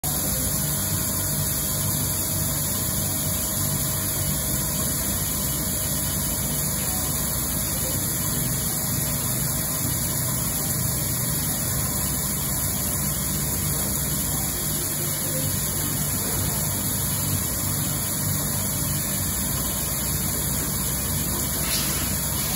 Milling chrome steel on an Okuma Multus from Veenstra Technology - second production step
Second production step in milling chrome steel clamping jaws on an Okuma Multus from Veenstra Technology. These clamping jaws are applied for swaging units for swaging and thread cutting of drill pipes